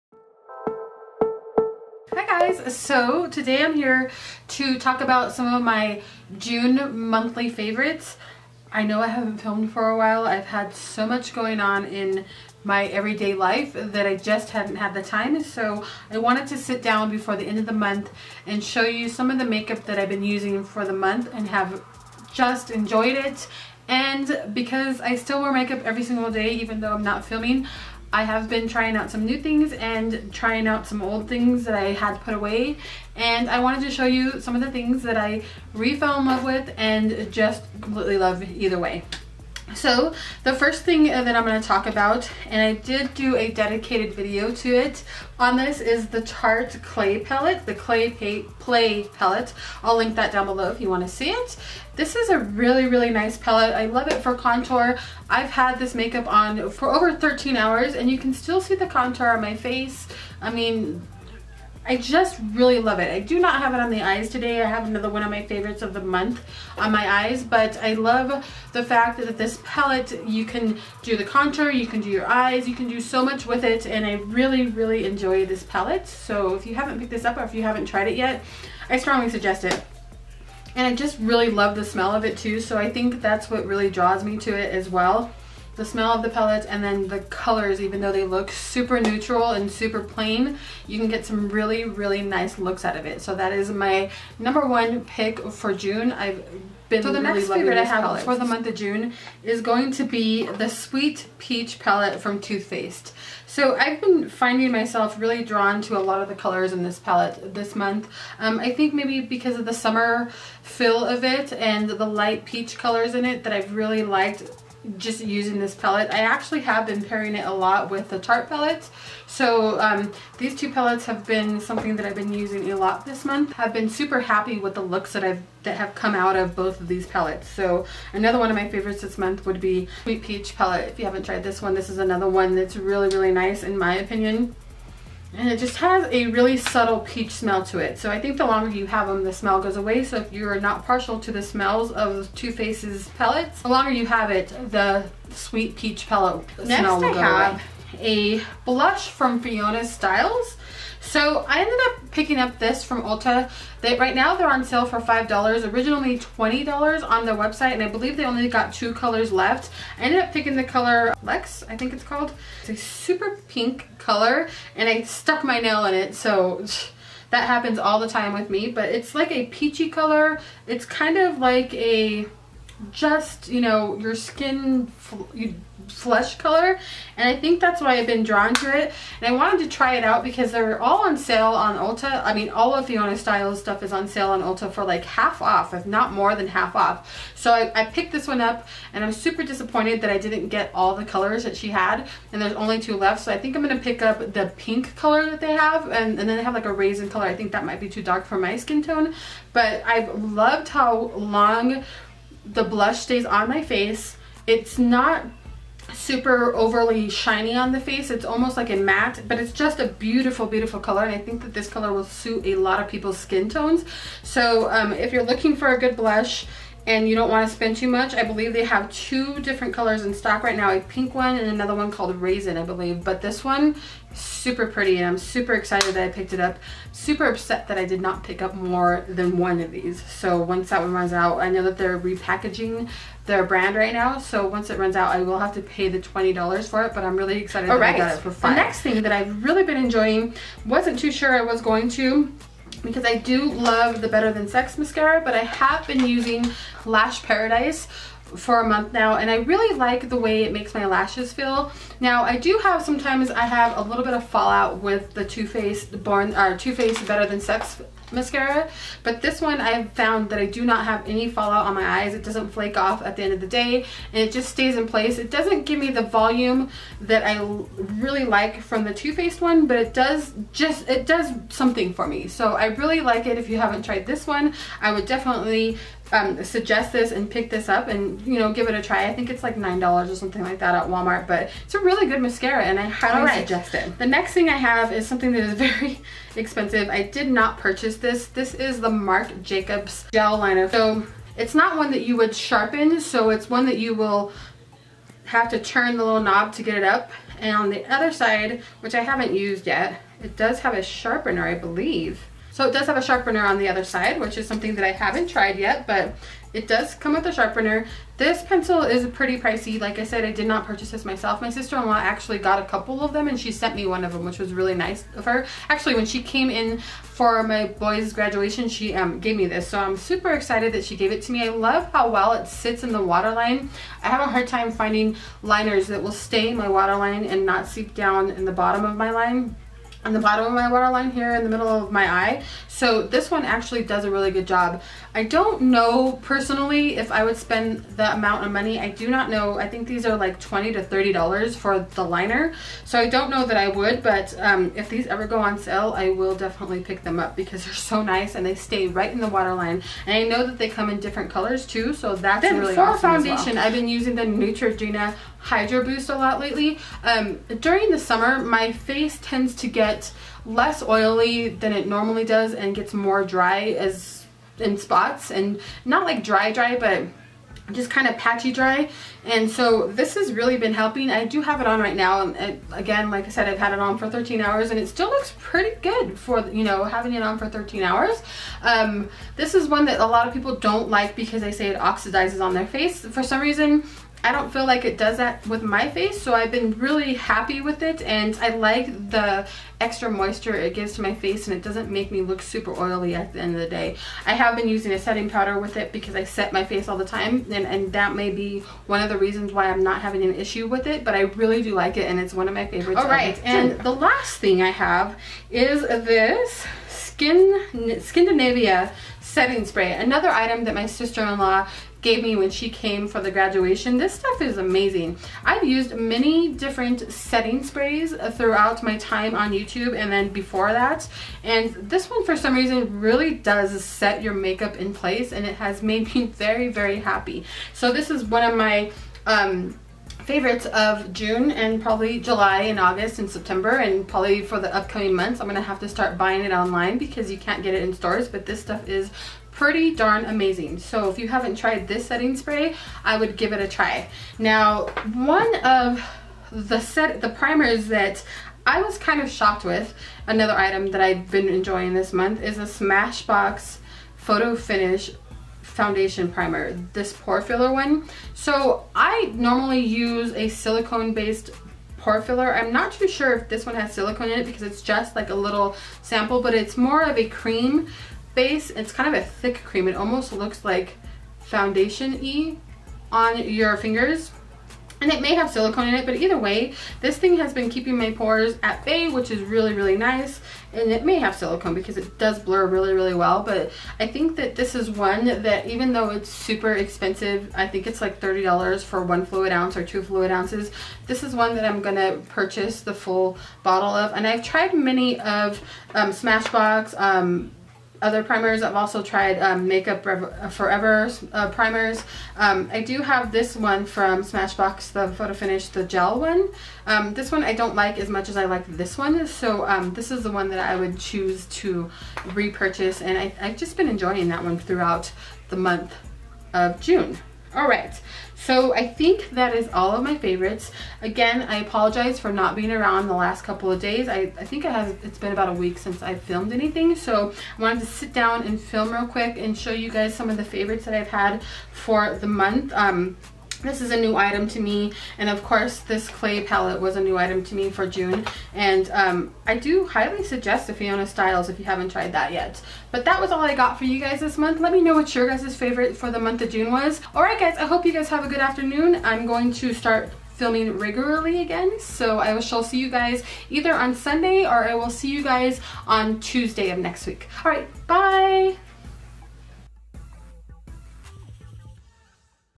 JUNE 2017 MAKEUP FAVORITES | Sabrina The MakeupMom
Today's video is my favorites for the month of June. I've really enjoyed using all this makeup and I hope you all will give it a try.
My monthly Favorites.

TARTE
Clay Play Face Shaping Palette $46

TOO FACED
Sweet Peach Eyeshadow Palette $49

FIONA STILES
Soft Cheek Veil $5.00

L'ORÉAL
Voluminous Lash Paradise Mascara $9.99

Marc Jacobs Beauty
Highliner Gel Eye Crayon Eyeliner $25

NEUTROGENA
Hydro Boost Hydrating Tint $14.99

SMASHBOX
Photo Finish Foundation Primer Pore Minimizing $39.00

Skindinavia
The Makeup Finishing Spray - Bridal (4 fl oz.)  $29.00

other makeup channels I think you would enjoy
Aamyie Ecker